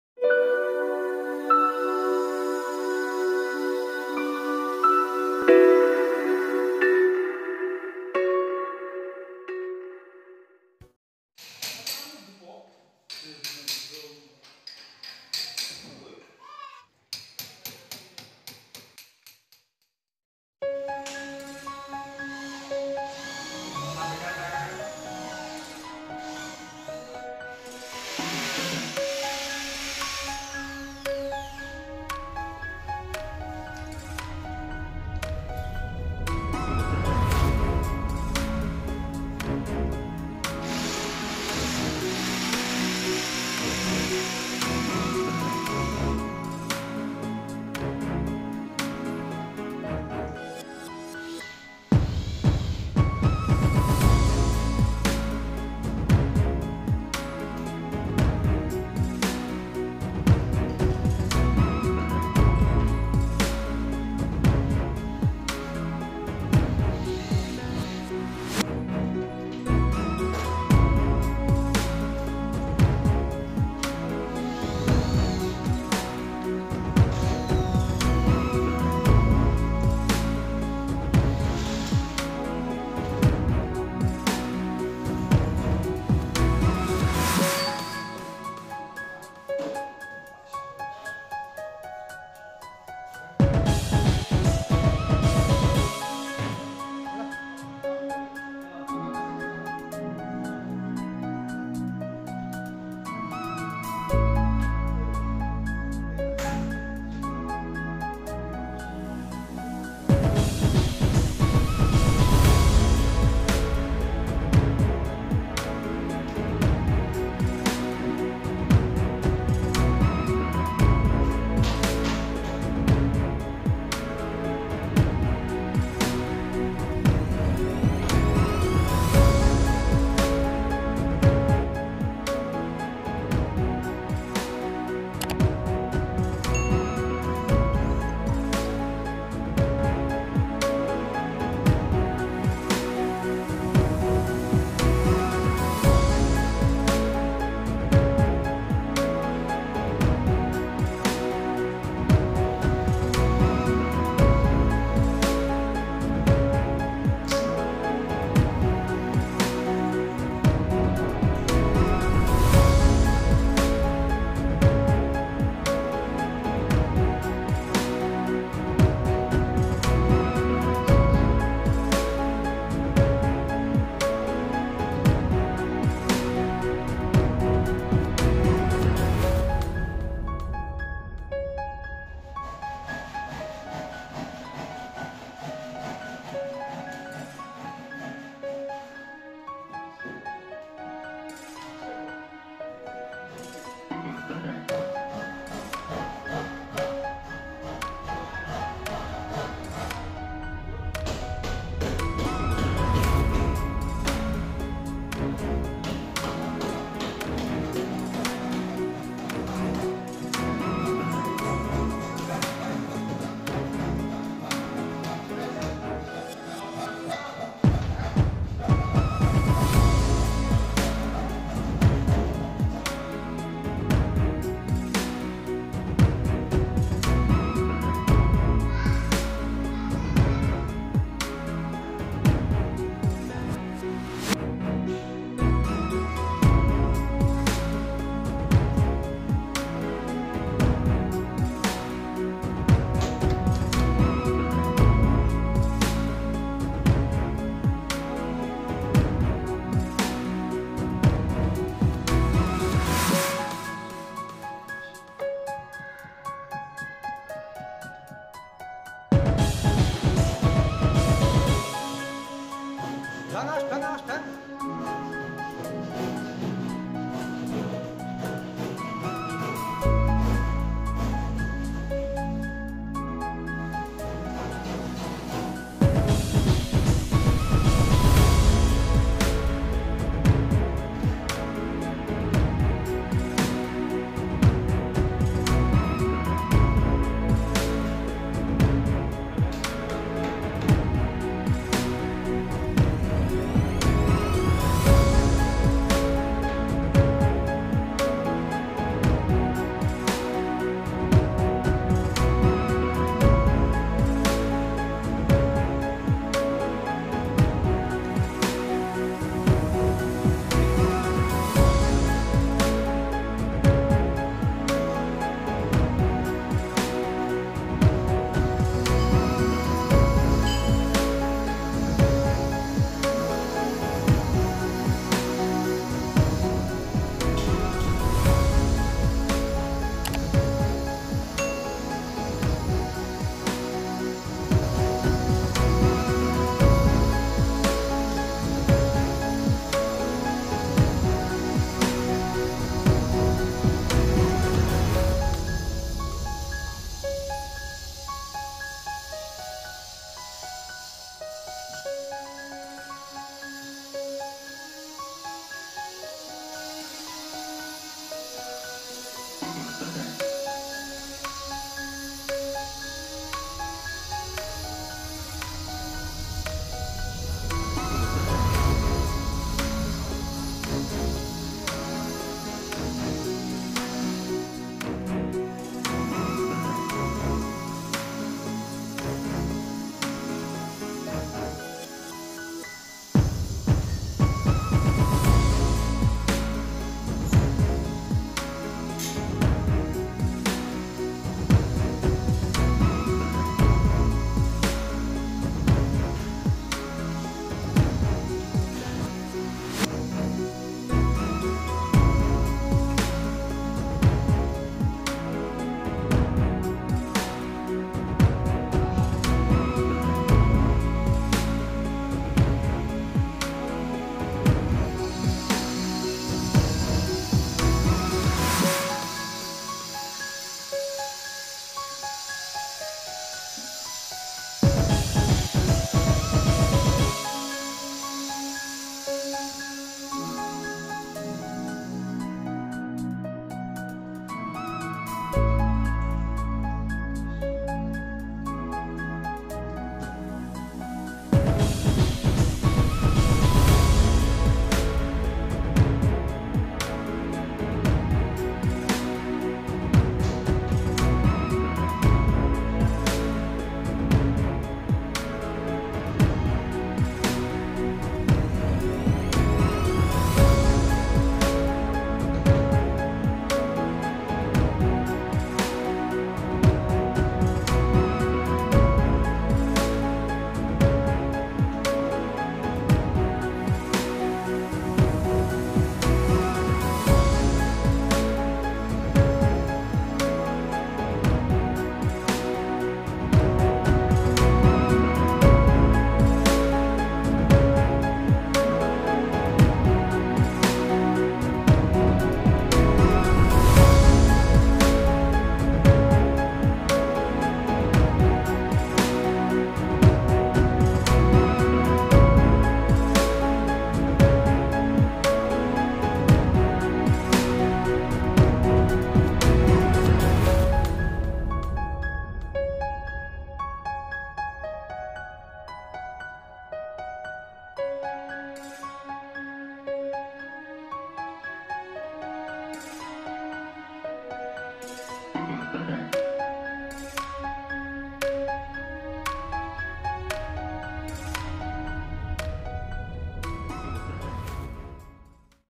Vlog kitchen set #4 || pemasangan Kitchen set di Kedungdowo Kudus
Kitchen set kali ini merupakan paket lengkap, yakni lengkap menggunakan minibar dan juga box kulkas.
Selain itu pada kabinet bawah menggunakan full kabinet serta menggunakan top table granit warna hitam.